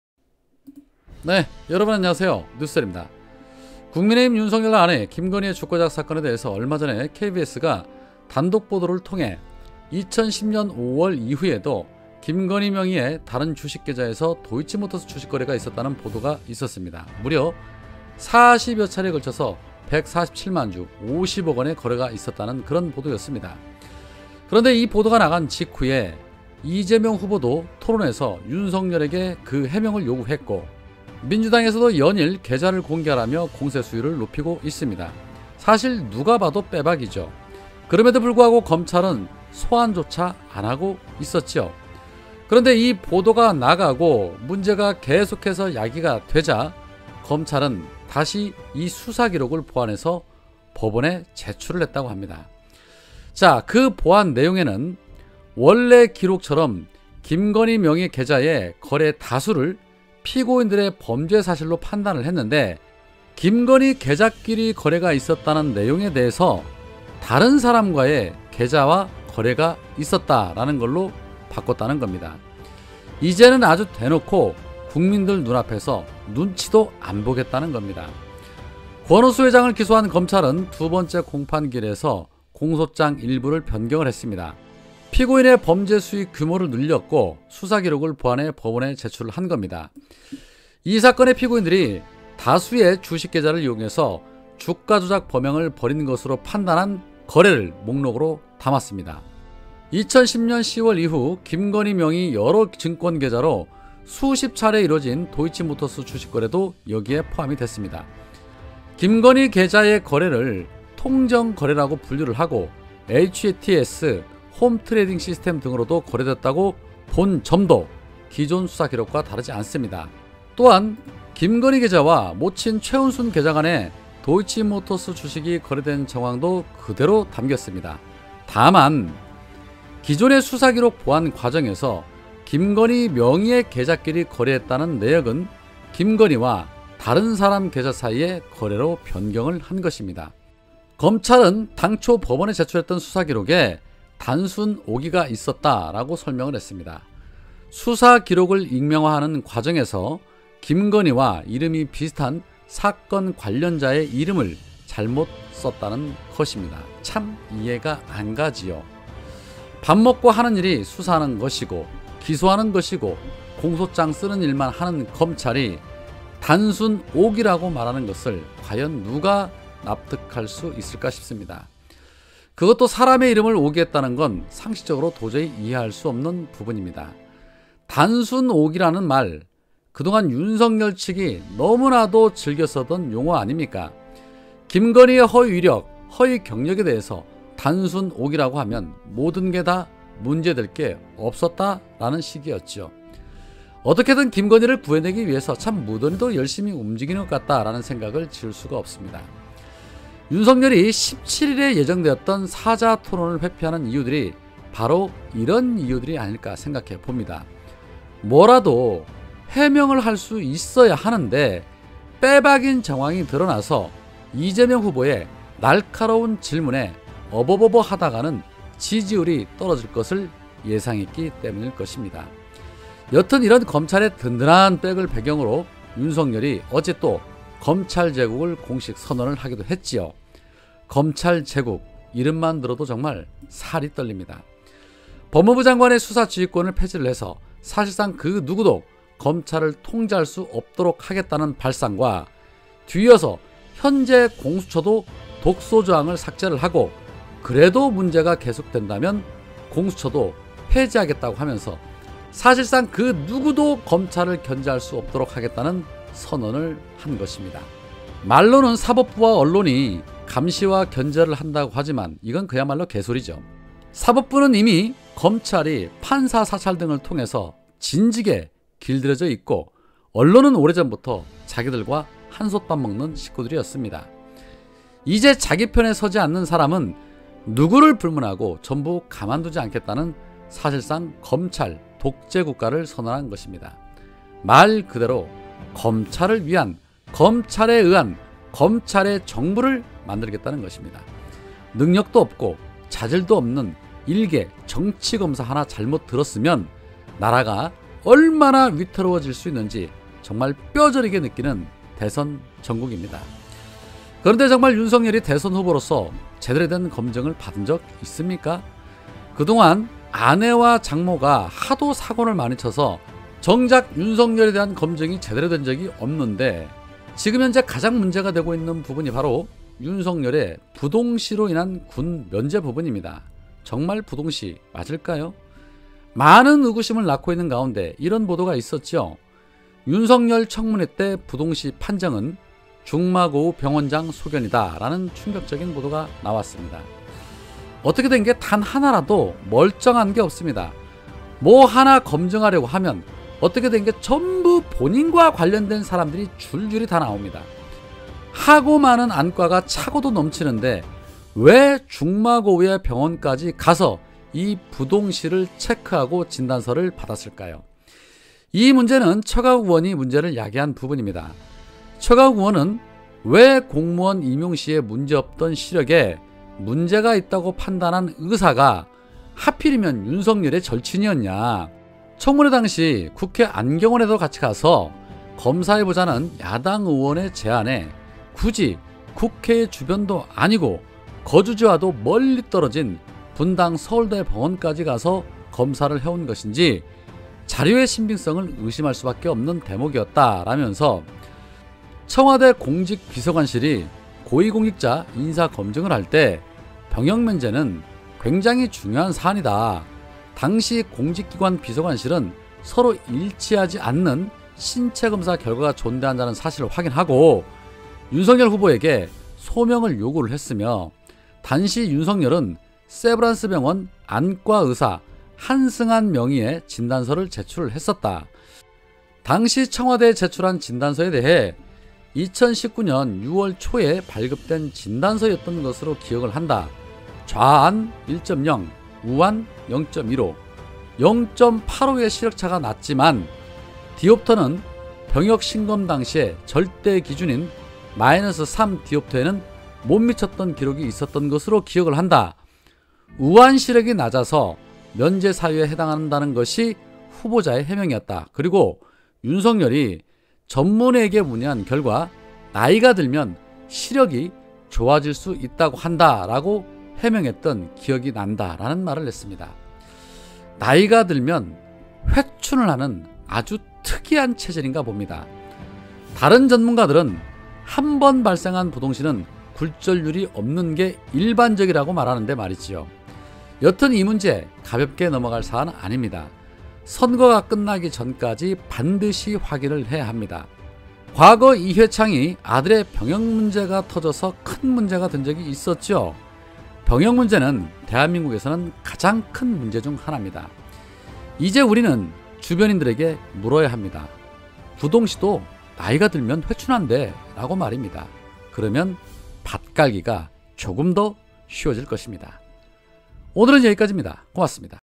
검찰 김거늬 공소장 급 변경! 단순오기? 일반거래 둔갑!
검찰개혁,사법개혁,언론개혁을 적극 응원합니다..